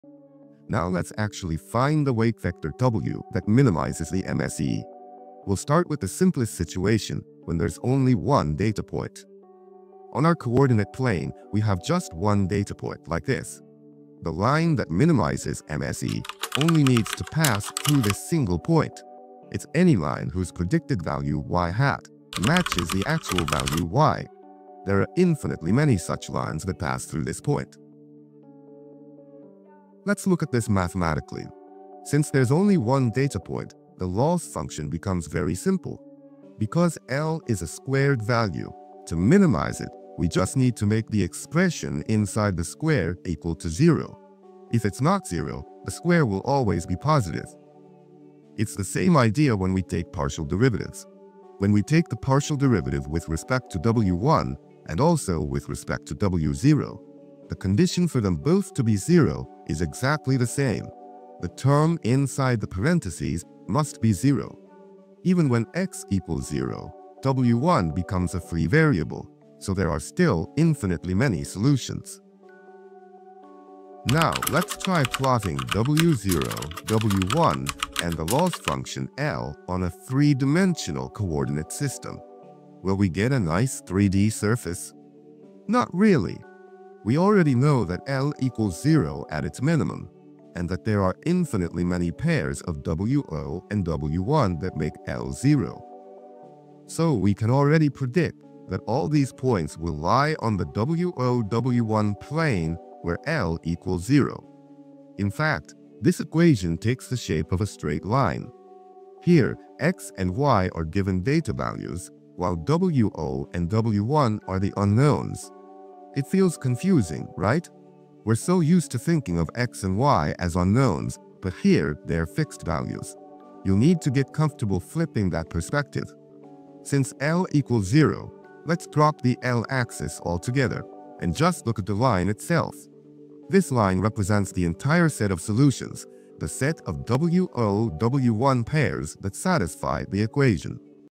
Visualizing MSE with a Single Data Point: Why the Loss Can’t Pick One Line
When we only have one data point, the line that minimizes the Mean Squared Error (MSE) isn’t unique — any line passing through that point gives zero loss.
In this short, we visualize why there are infinitely many solutions for (w₀, w₁), and how the loss function forms a flat line in 3D space.
It’s a simple but powerful example of what MSE really means in linear regression.

All animations in this video were made with ManimGL and ManimCE, an open-source animation library built in Python. You can find more information about the project